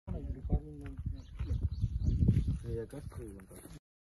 Telangana : 3 Acre Of Agricultural Land For Sale Located At Ananthasagar, Medak District
3 Acre of Agricultural Land for sale located at Ananthasagar, Medak District, Telangana. Price - 65 lakhs per Acre. It has poultry sheds with a build-up area of 25000 sq feet land and you will get 400000 rent per year. The land is situated 2 km from the highway.